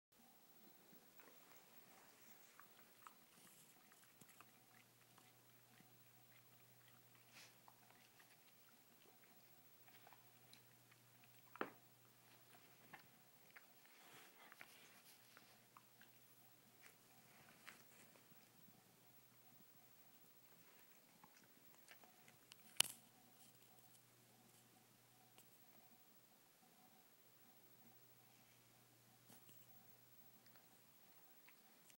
A Hoo & his pink peep.
Daddy got Hooey a pink stuffed peep for Easter.